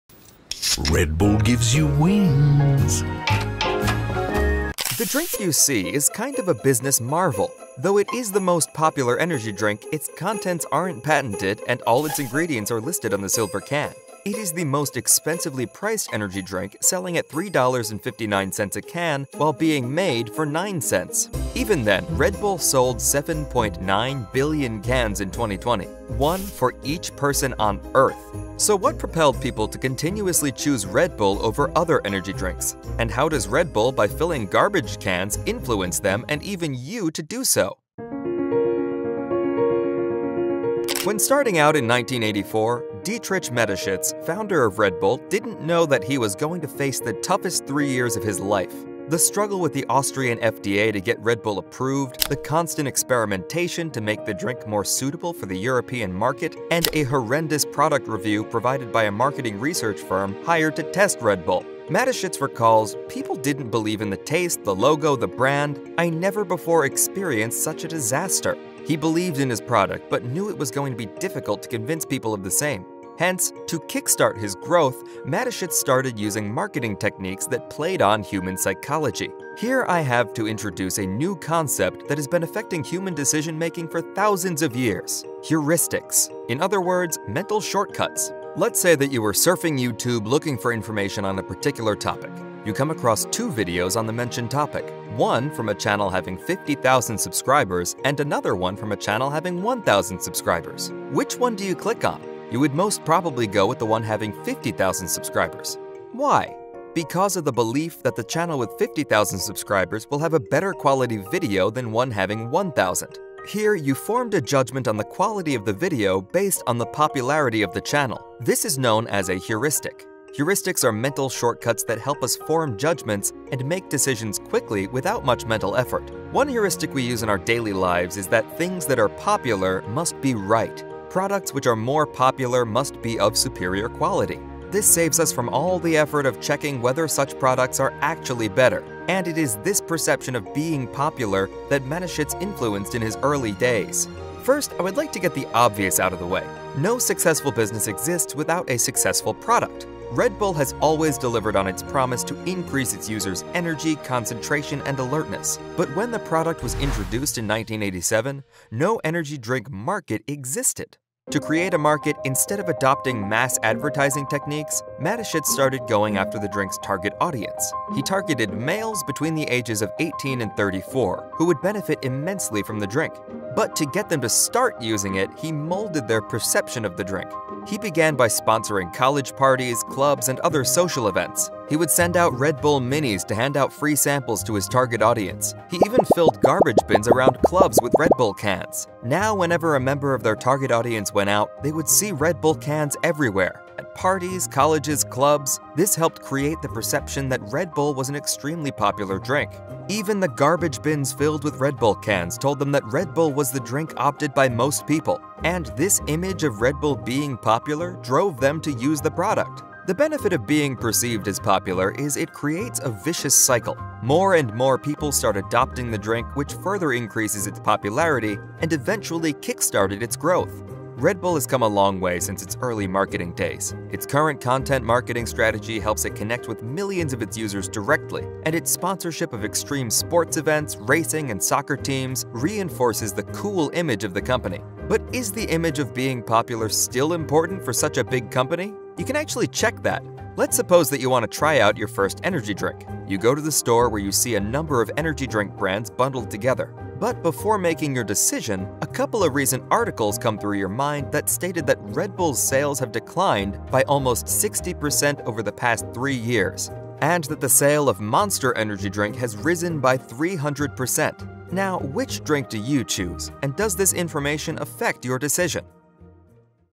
Understanding the Psychology behind Red Bulls WEIRD MARKETING STRATEGY
Red Bull, the most popular energy drink, sold 7.9 billion cans in 2020, one for each person on earth. However, when the company originally started out in 1984 no energy drink market existed. To make matters tougher, people didn't believe in the product at all. Still, Dietrich Mateschitz, founder of Red Bull, had confidence in his product and started adopting marketing techniques that played on human psychology to kickstart the companies growth. And following this approach, he decided to fill garbage bins with Red Bull cans.

00:00 - Intro
00:41 - The Beginning
01:20 - The human mindset
02:40 - The tactics begin
03:54 - Is it still important?